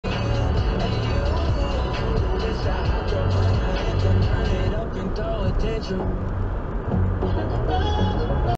Silverado Overtakes Bike on a Blind Curve
Please wait for a straight road to pass